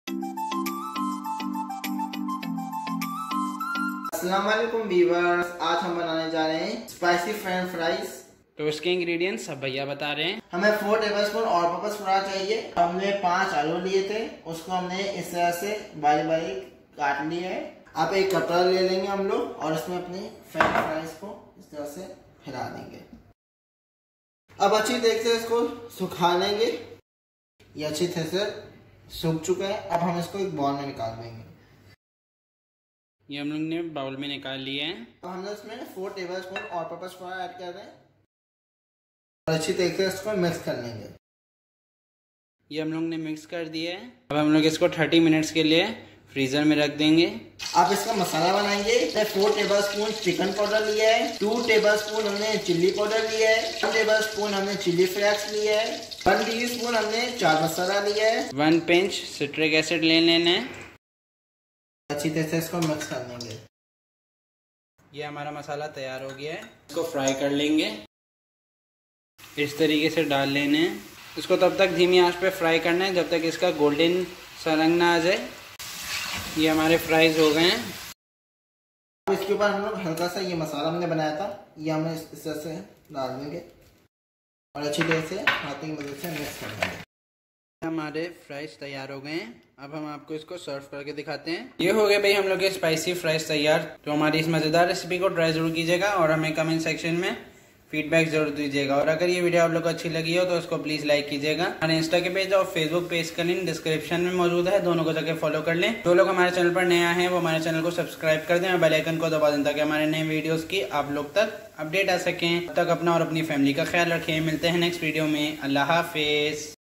How To Make Spicy French Fries | Homemade Masala | Bihaar Cooks | Easy Recipe | Fast Food | Yummy
INGREDIENTS:-
1. TAKE 4 tbsp ALL PURPOSE FLOUR.
2. TAKE 5 POTATOES AND CUT IT.
FOR MASALA:-
1. TAKE 4 tbsp CHICKEN POWDER.
2. TAKE 2 tbsp CHILLI POWDER.
3. TAKE 1 tbsp CHILLI FLAKES.
4. TAKE 1 tsp CHAAT MASALA.
5. TAKE 1 PINCH CITRIC ACID.

Musician: @iksonmusic